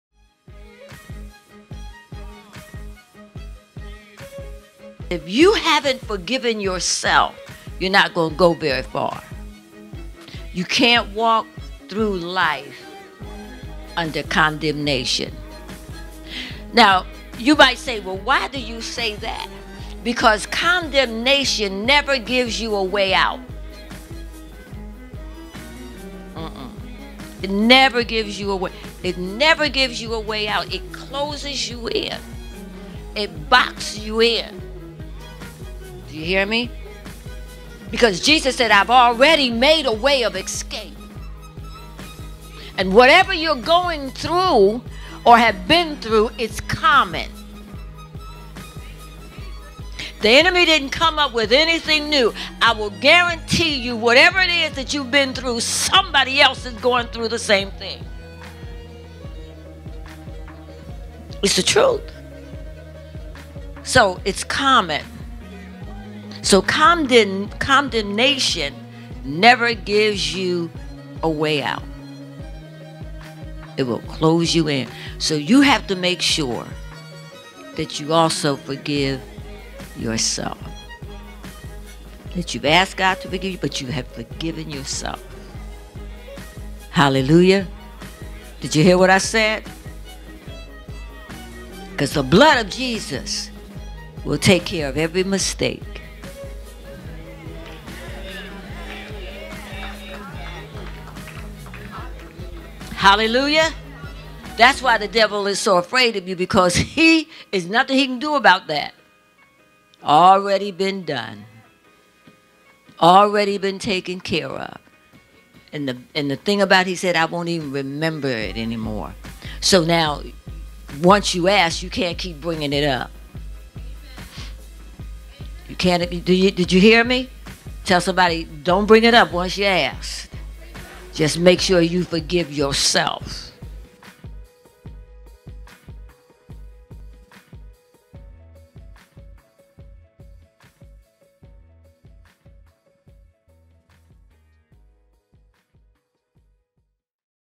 Forgive Yourself - Dr. Joyce E. Scott
Sometime the hardest thing is the easiest thing to lead to true joy.  Dr. Scott can help you walk in the place of forgiveness .  Here is just a taste.